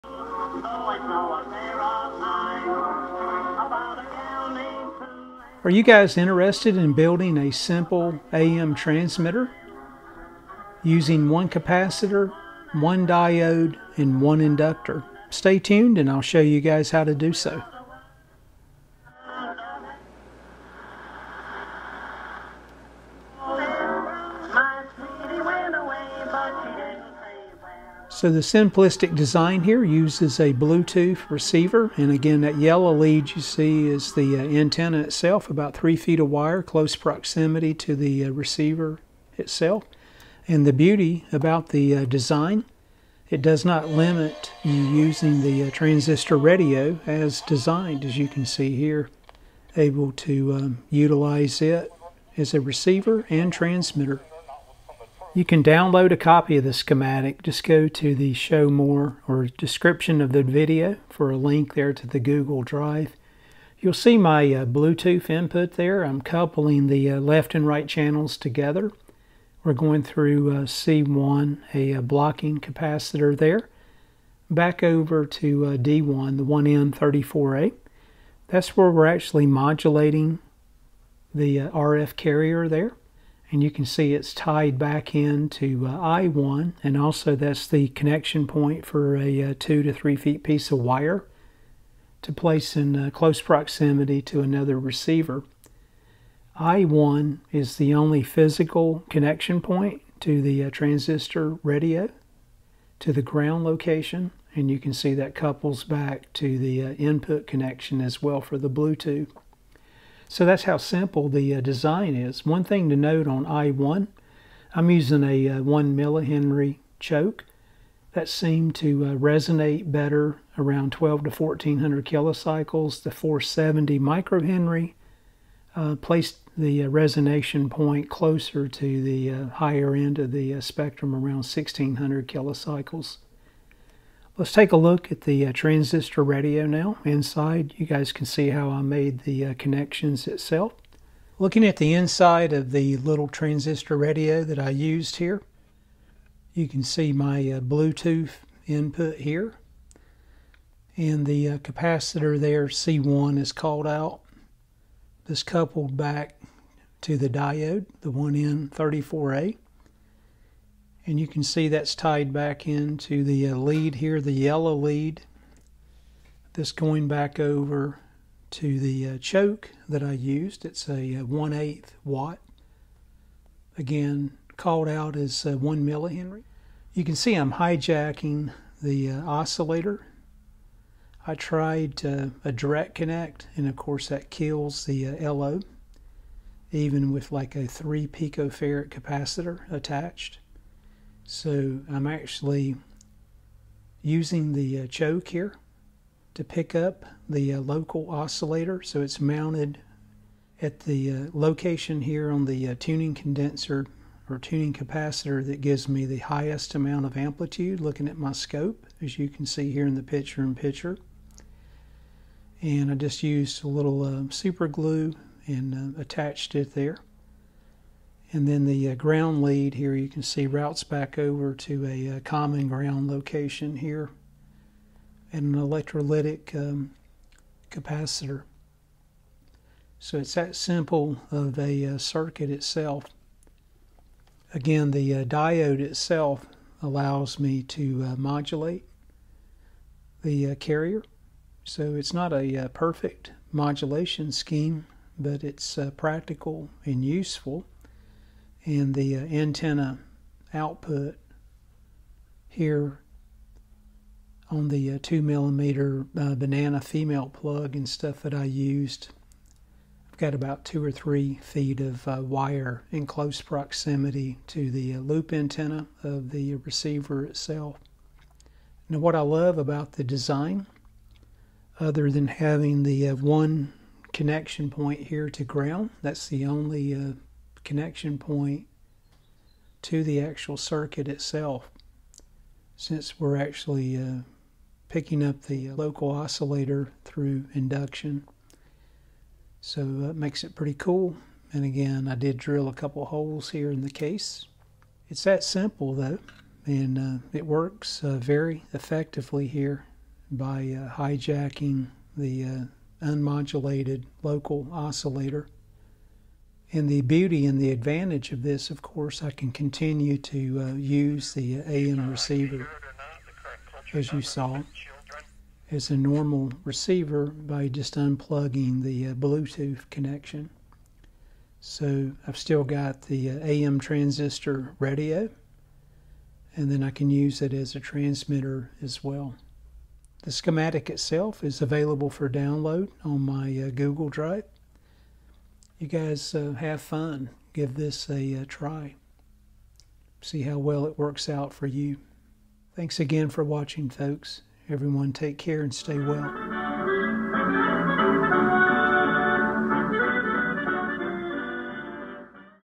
How to Build an AM Transmitter w/ one Capacitor, Diode & Inductor
A simple, but effective close proximity AM Broadcast Transmitter you can build using a Transistor radio and only three components. My build covers 995 kHz to 1700+ kHz.

Check out v2.0 as well that attaches directly to the local oscillator.